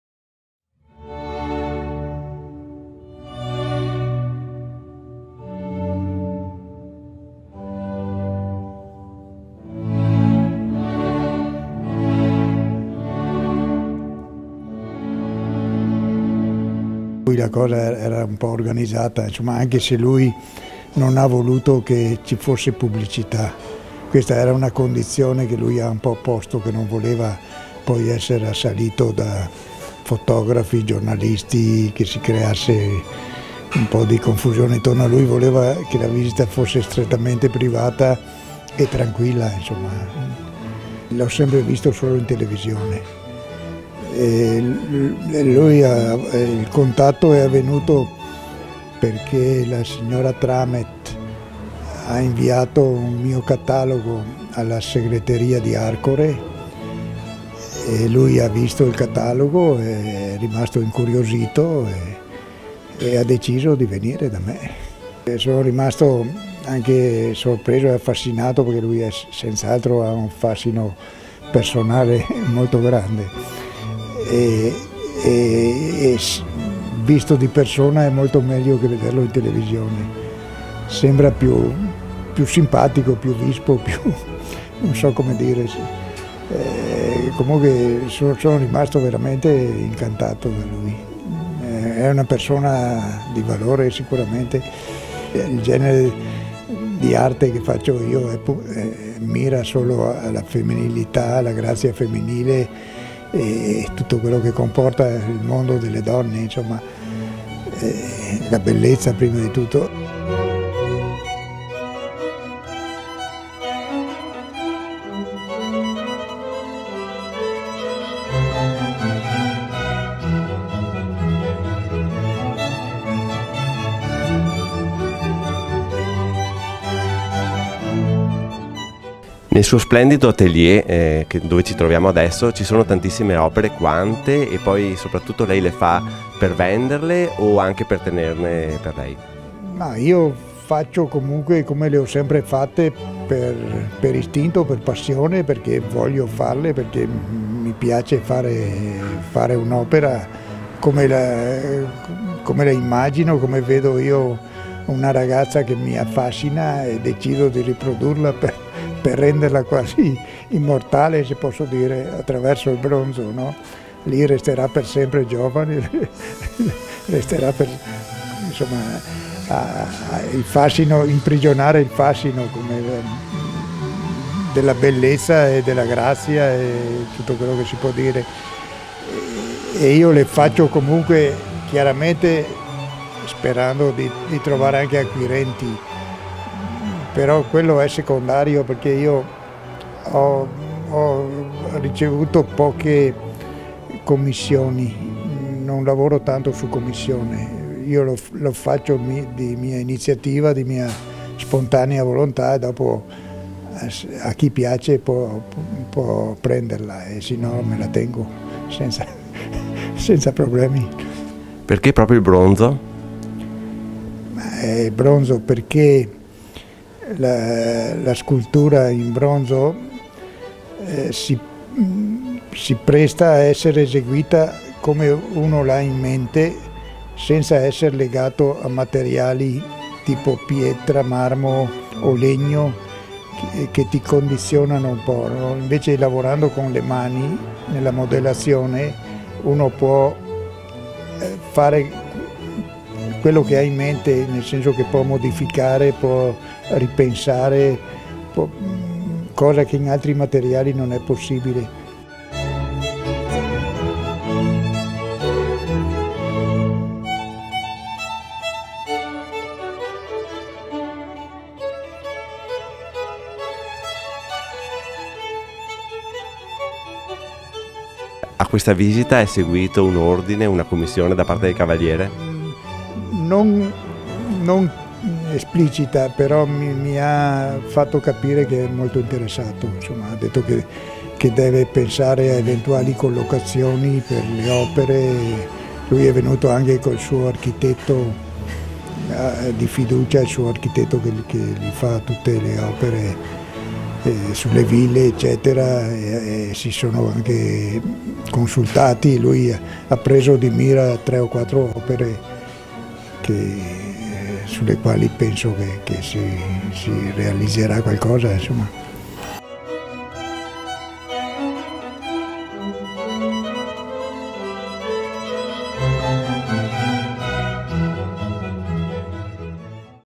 L'artista Valerius il motivo del “blitz” di Berlusconi a Pieve di Soligo
Un’artista abile a immortalare la bellezza femminile nelle sue opere in bronzo non poteva non attirare l’attenzione di Silvio Berlusconi. E’ Valerio De Marchi in arte Valerius, il maestro scultore pievigino, il vero motivo del blitz di ieri, mercoledì 12 settembre, nel Quartier del Piave dell’ex premier Silvio Berlusconi.